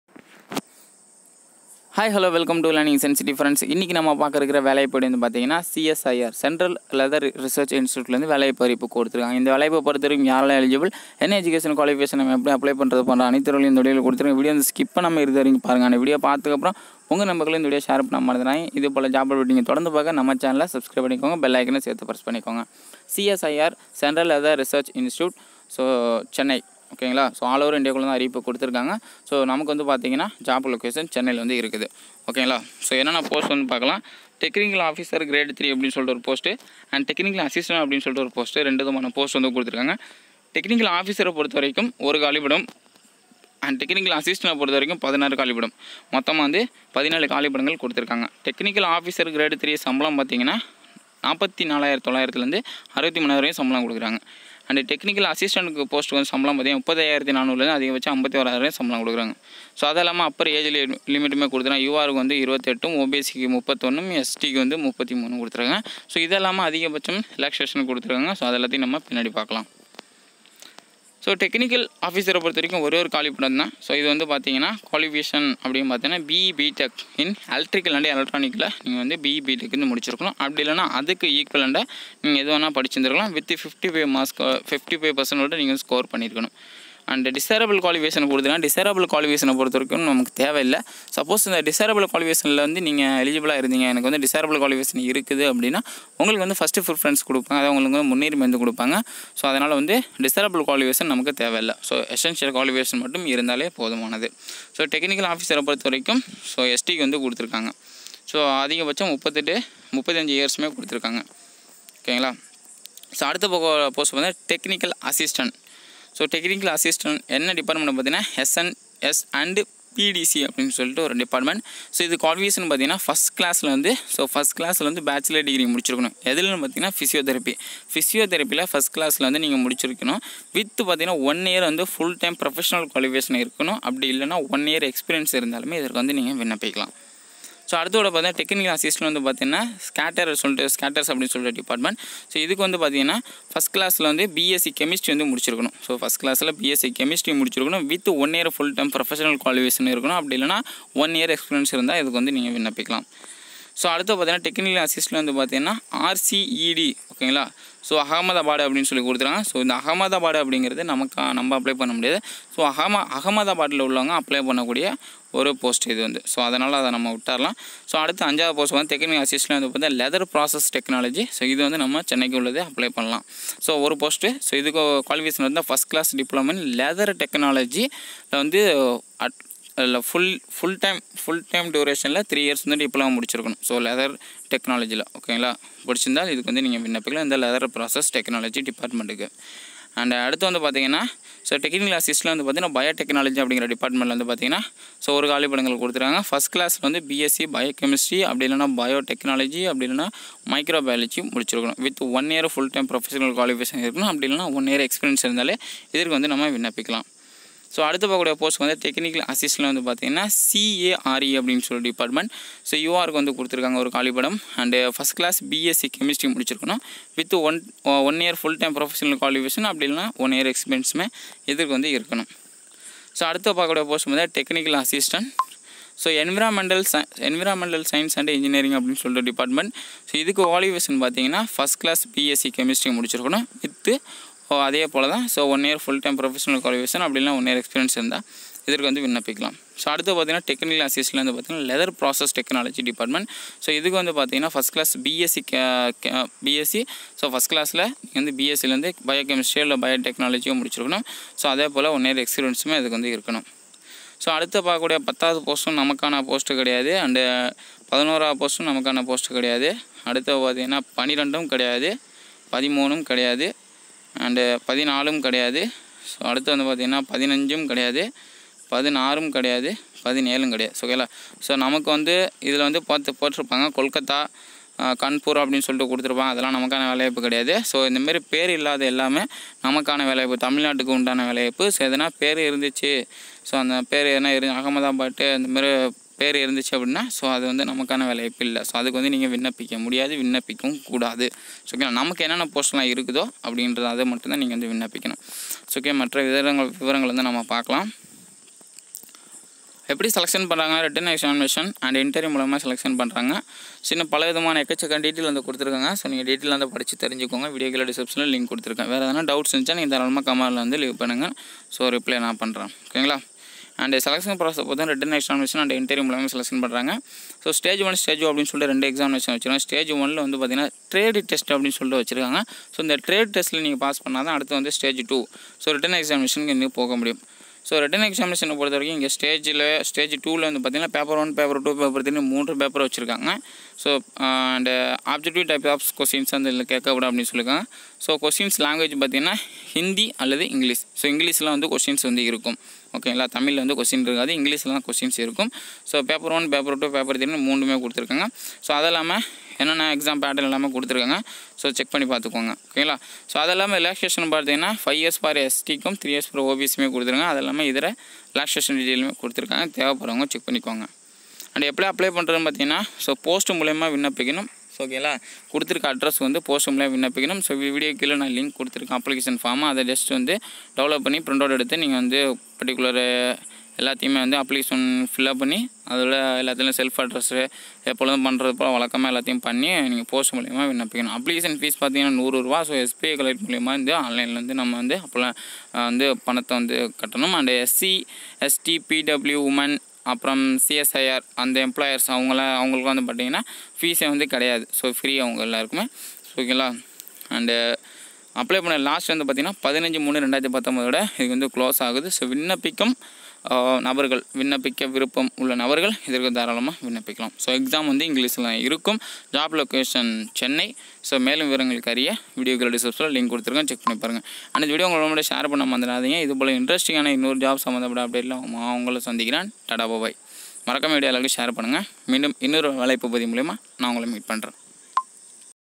diploma & degree படித்தவர்களுக்கான அசிஸ்டண்ட் வேலை || CSIR-2019 || salary 63,000 || apply now...
notification link.